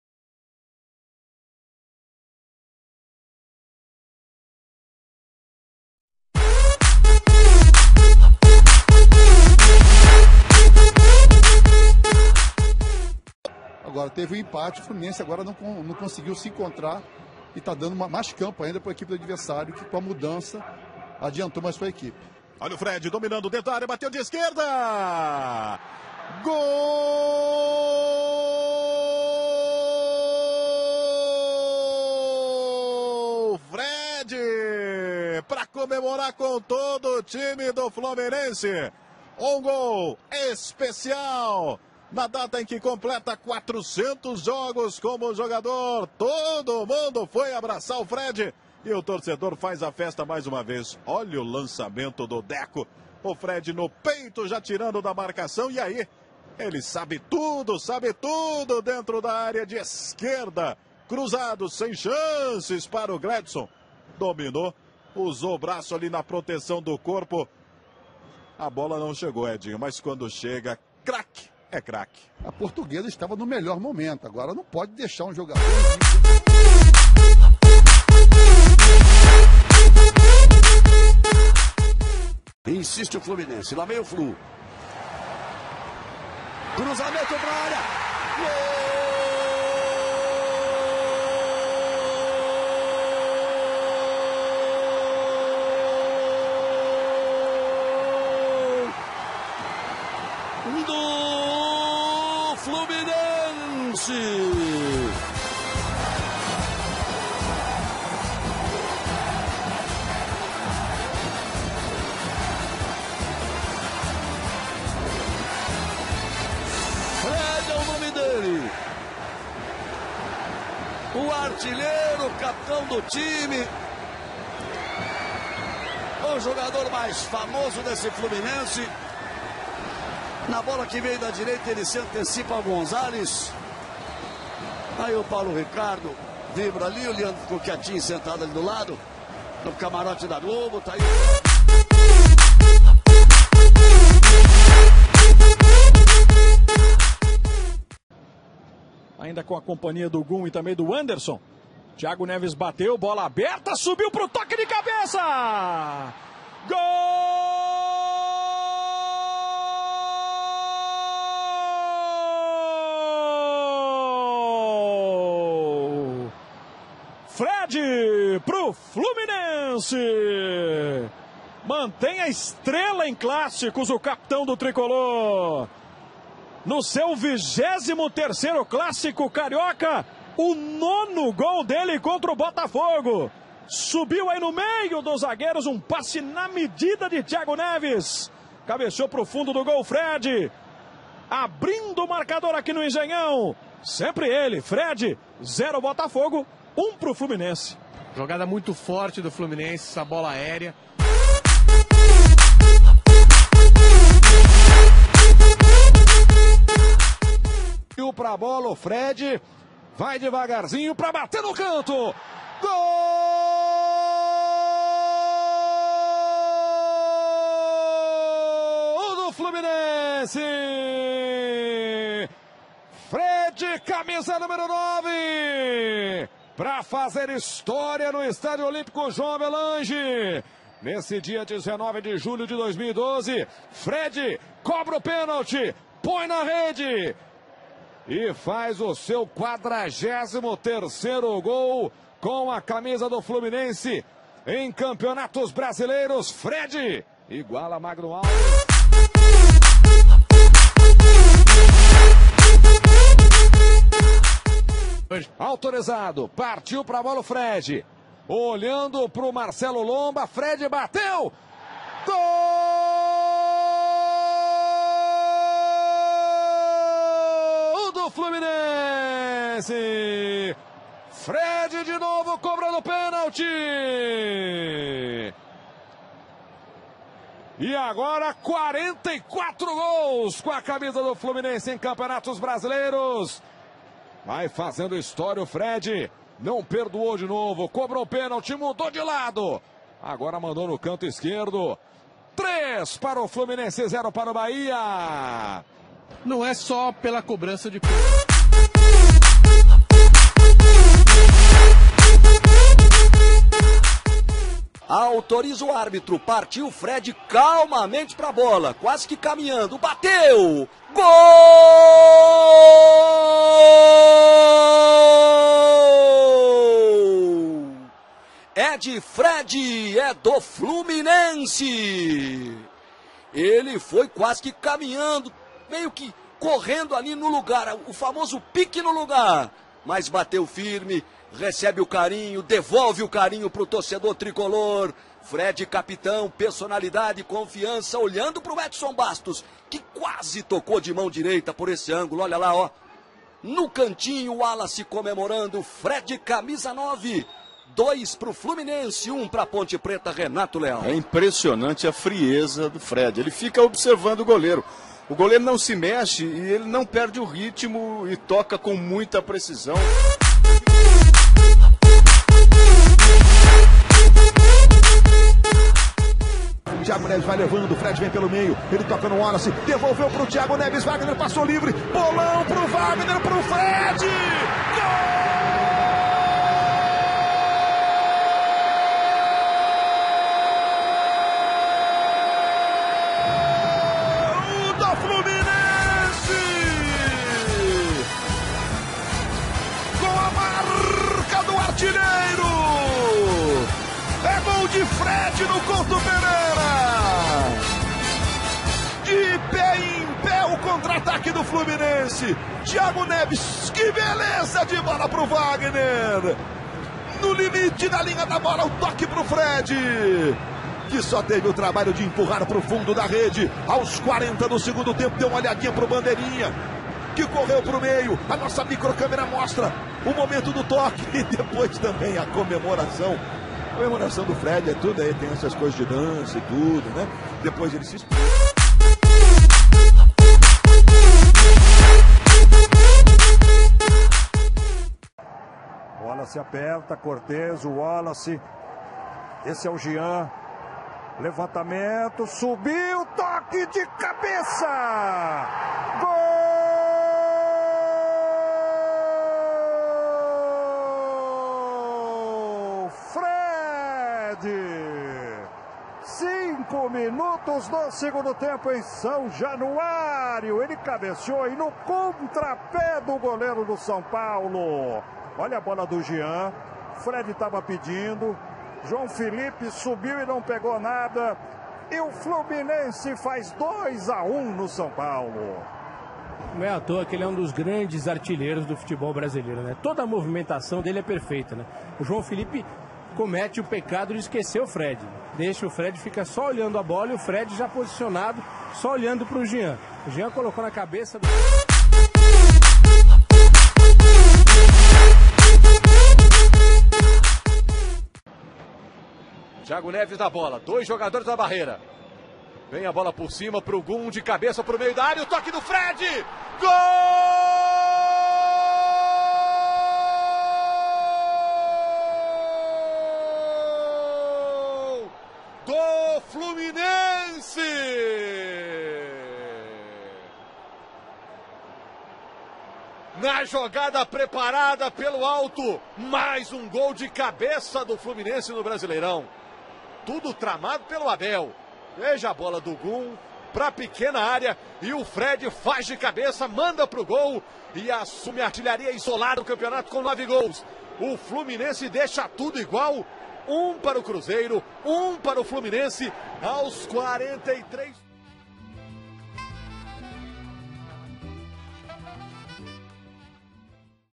Fred #9 - Fluminense - 2012
Fluzao ! Tous les buts en championnat du goléador Fred !

A suivre ...